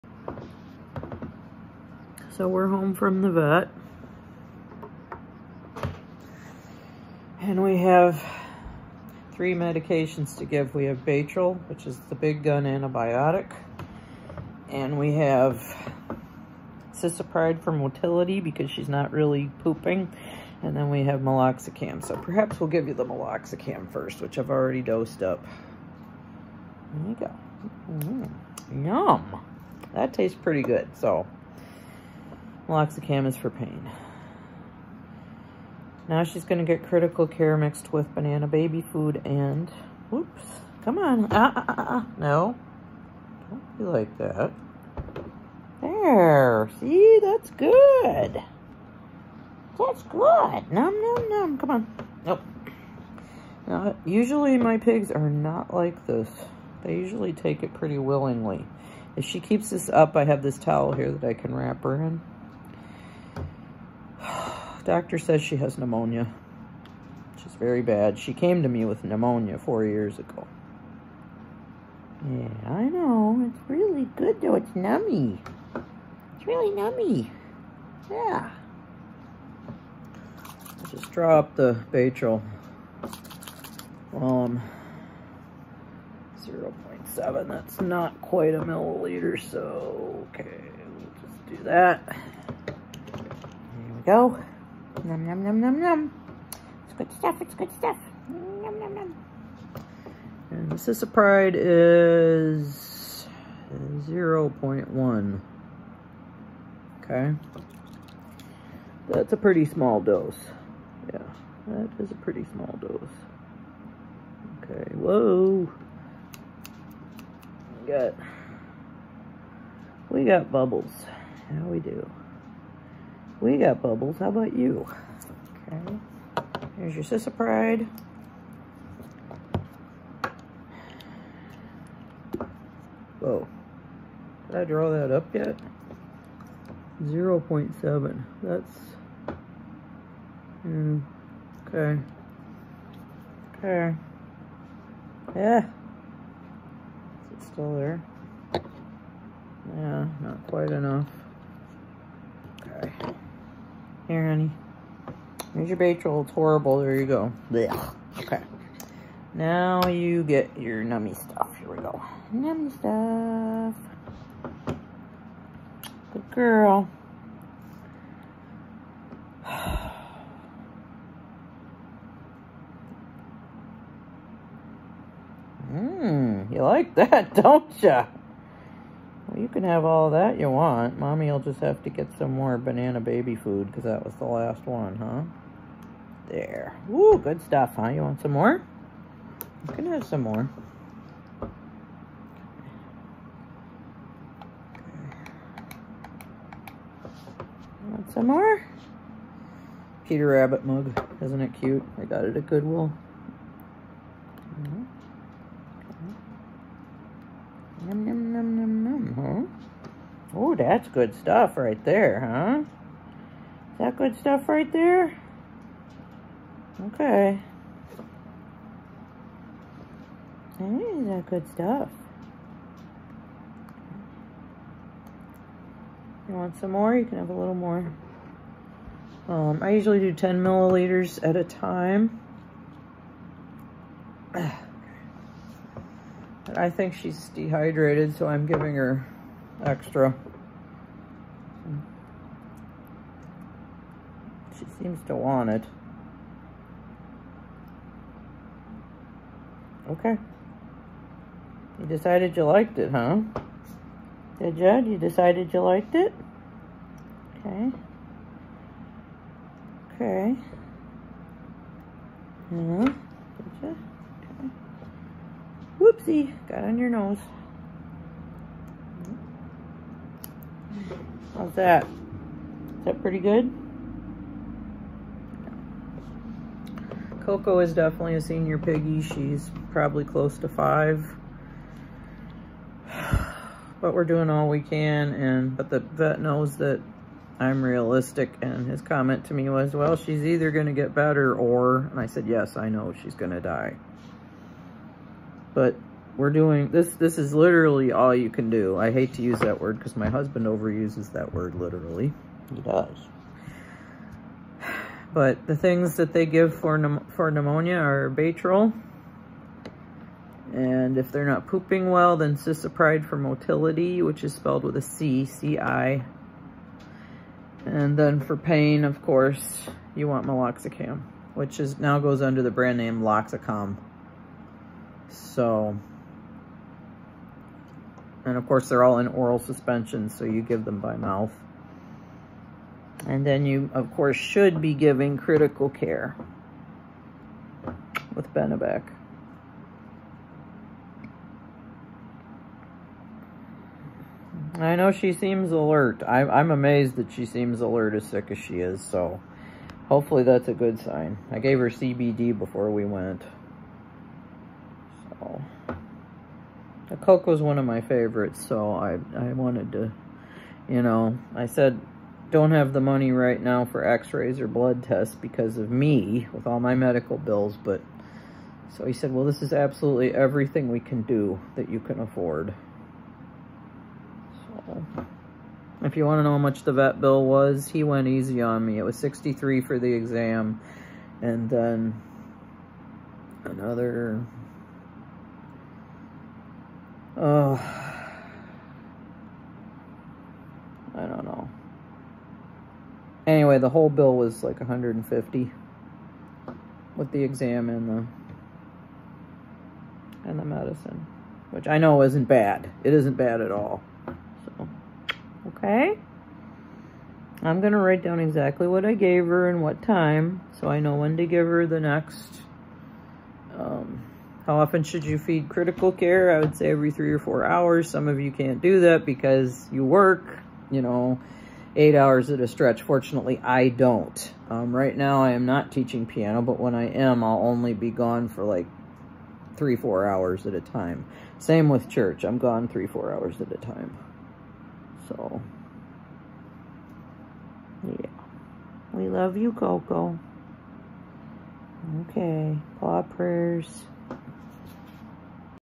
Senior piggie with Pneumonia, how to medicate and give supplemental food
Coco, my￼ senior sow, presented with crunchy eyes and nose this morning. I did not wait and called three vets until I was able to get in with my old country vet. I told him I was paying lots of bills on my own healthcare and didn’t want x-rays or blood tests so he gave me all the medicine that she can have. He was also realistic, she will either get better or die
As far as the dose of Baytril, 0.7 is not quite 0.1 on a 1 mL syringe so it is very small. Cisapride is exactly 0.1 so these are very tiny doses.
￼Then give critical care or Sherwood recovery food mixed with banana baby  food and benebac.
Feed 5 to 10 ccs every 3 to 4 hours to keep Piggies hydrated .￼
When a  piggy has pneumonia…You must get Baytril on board as soon as possible. This is very serious.